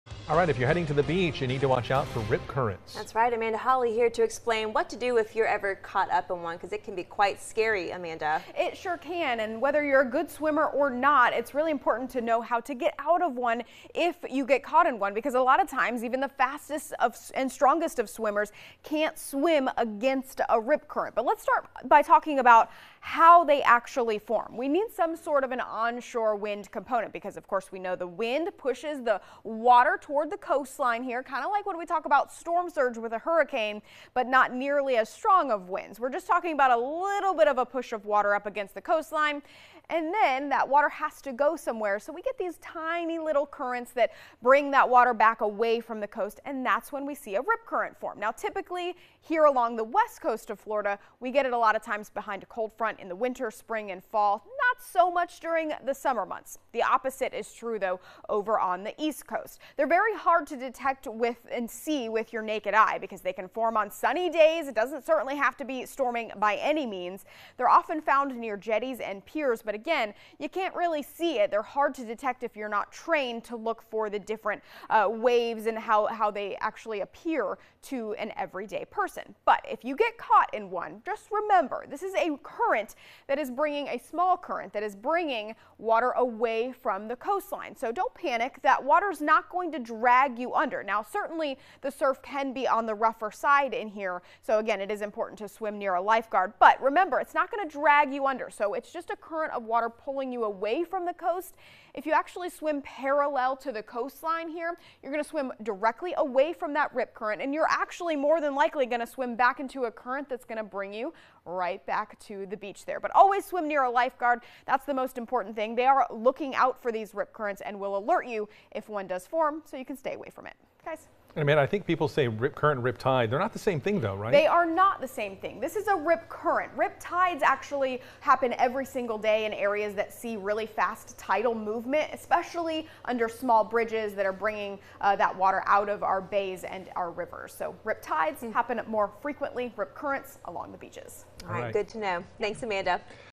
Severe Weather Awareness Week: Rip Currents
Severe Weather Awareness Week: Rip Currents: Meteorologist Amanda Holly details how to escape a rip current and the different between a rip current and a rip tide.